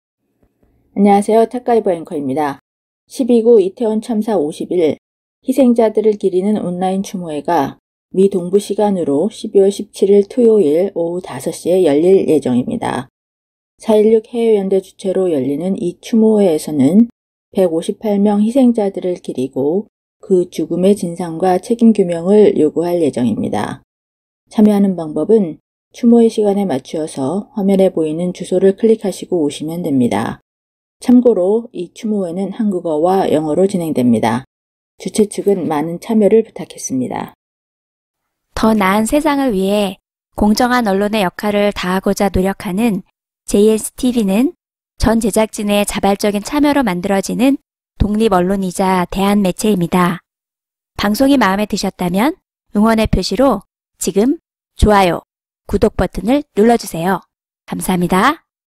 전 세계 한인들 참여하는 이태원 참사 50일 온라인 추모회 열린다.
-미 동부 17일 오후 5시, 한국 18일 오전 7시,
-줌 회의 아이디: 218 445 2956, 비번: 1004